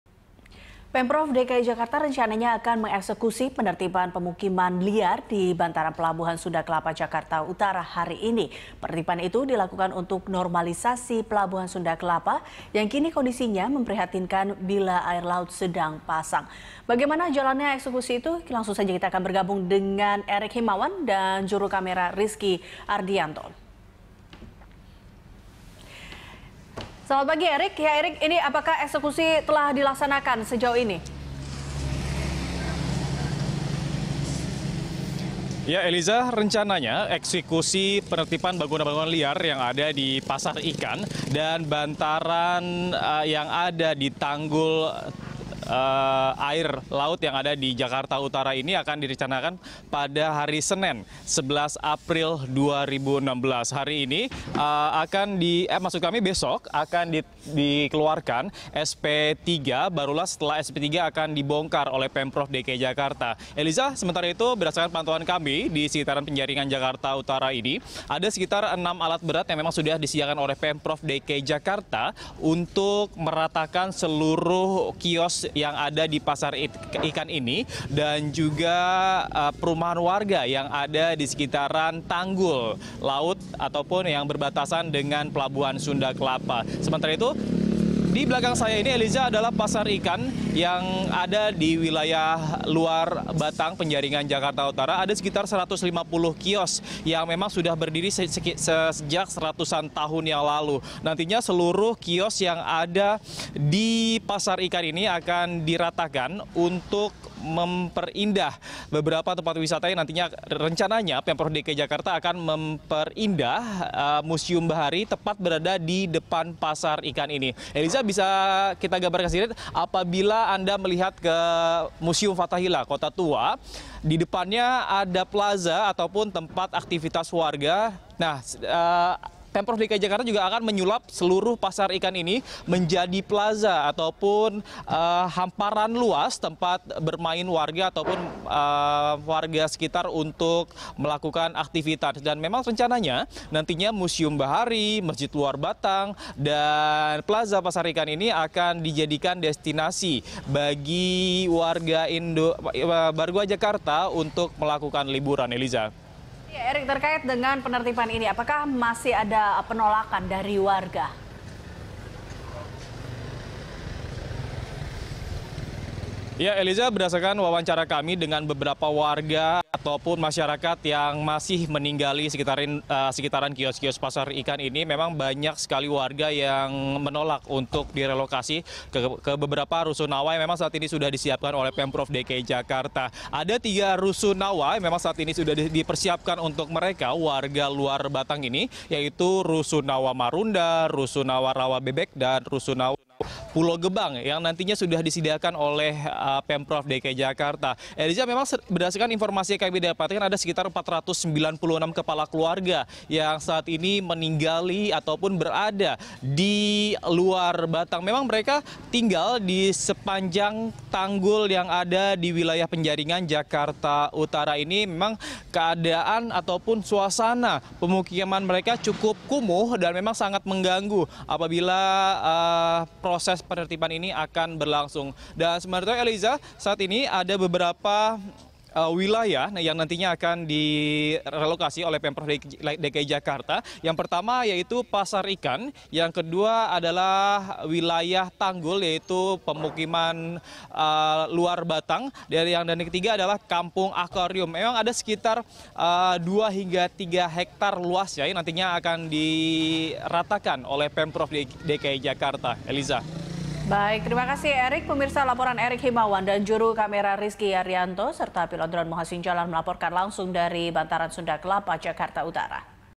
Bangunan Liar di Sekitar Pelabuhan Sunda Kelapa Akan Ditertibkan
Pemerintah Provinsi DKI Jakarta rencananya mengeksekusi penertiban permukiman liar di Bantaran Pelabuhan Sunda Kelapa Jakarta Utara.

Facebook.com/BeritaSatuTV
Youtube.com/BeritaSatu